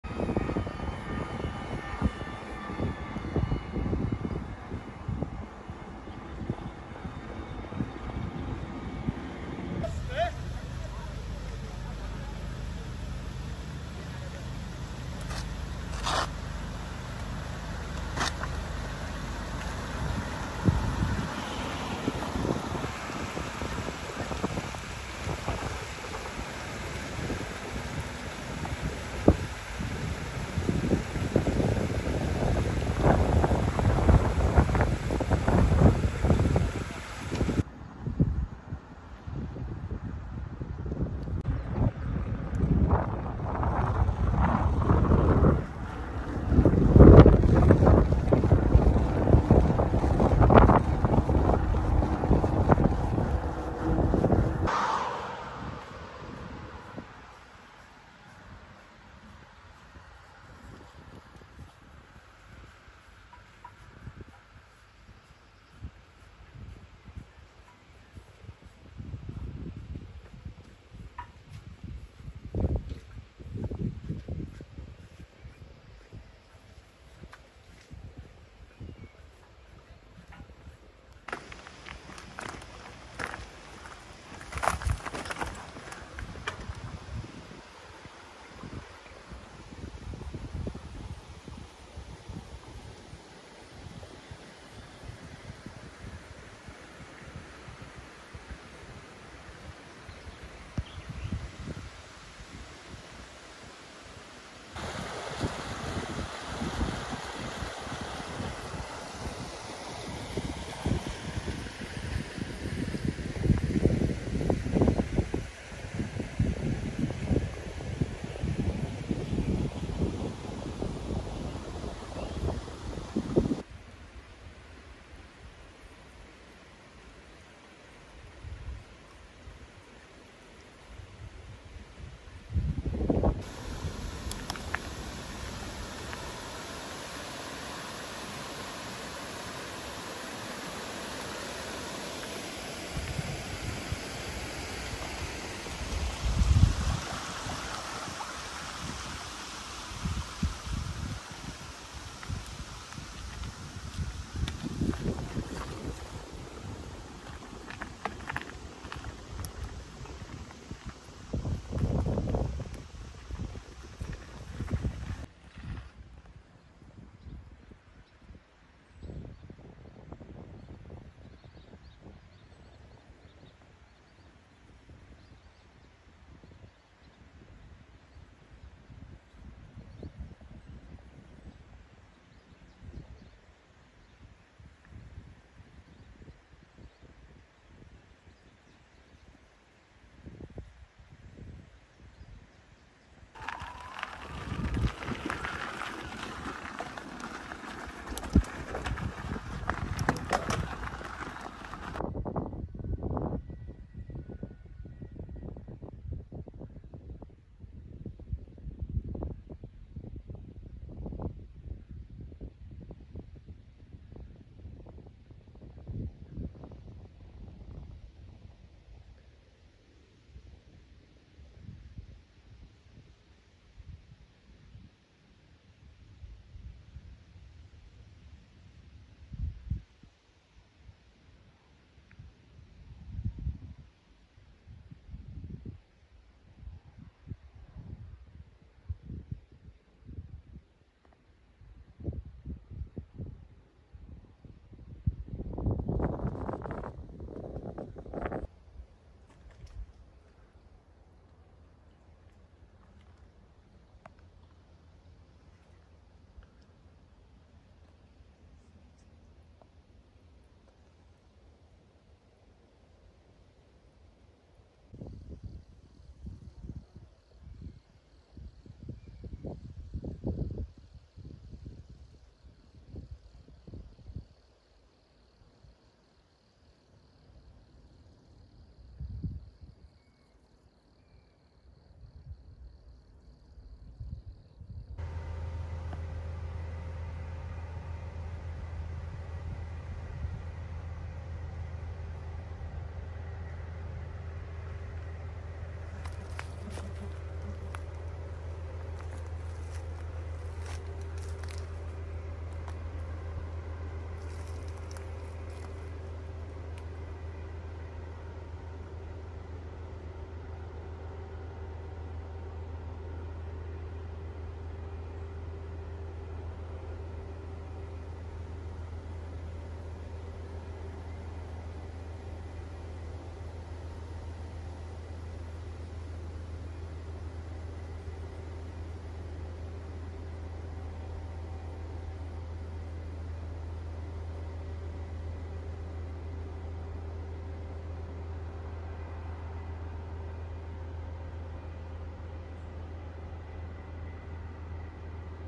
Die Region um Rheinbach Euskirchen Loch Steinbachtalsperre am 17.07.21
In Loch hat der "Schiefelsbach" heftig zugeschlagen.
Bilder vom "Schiefelsbach" werden gezeigt.
Gute Fernsicht auf Euskirchen und Umgebung.